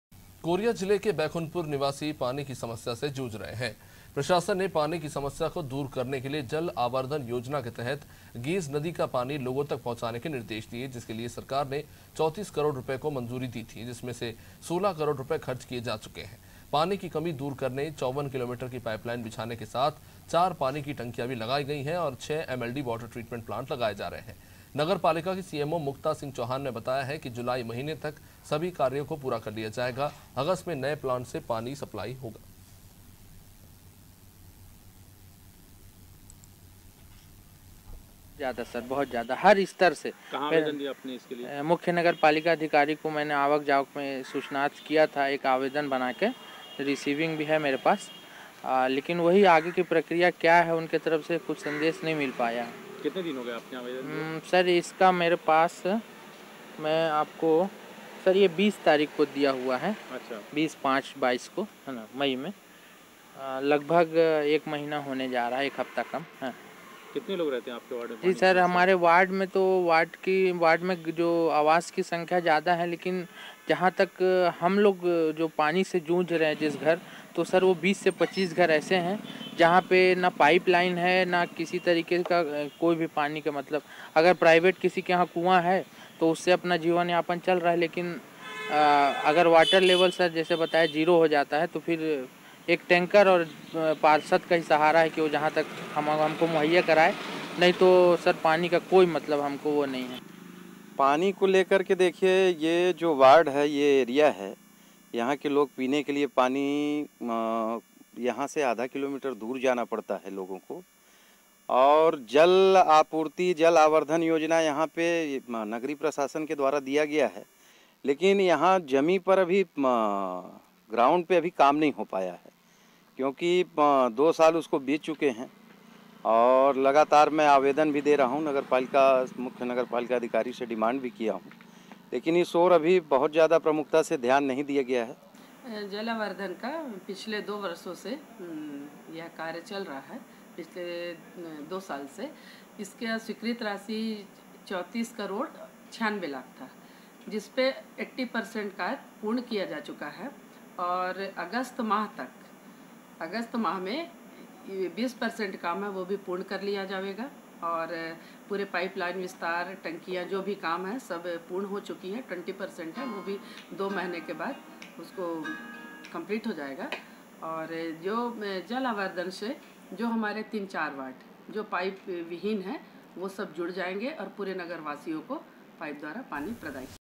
Koriya : पानी की समस्या से जूझ रहे लोग | Chhattisgarh News Hindi
कोरिया जिले के बैकुंठपुर के निवासी पानी की समस्या से जूझ रहे... प्रशासन ने पानी की समस्या को दूर करने के लिए.. जल आवर्धन योजना के तहत गेज नदी का पानी लोगों तक पहुंचाने के निर्देश दिए है... जिसके लिए सरकार ने 34 करोड़ रूपए को मंजूरी दी है... जिसमें से 16 करोड़ रूपए खर्ज किए जा चुके है... पानी की कमी दूर करने 54 किलोमीटर की पाईप लाइन बिछाने... के साथ चार पानी की टंकियां लगाई गई है... और 6 एमएलडी वाटर ट्रीटमेंट प्लांट लगाए जा रहे है... नगर पालिका की सीएमओ मुक्ता सिंह चौहान ने बताया की... जुलाई महीने तक सभी कार्यो को पूरा कर लिया जायेगा... अगस्त में नये प्लांट से पानी सप्लाई किया जाएगा... शहर में चार नई टंकियां लगाई गई है. अब सात टंकियों से पानी की सप्लाई की जायेगी.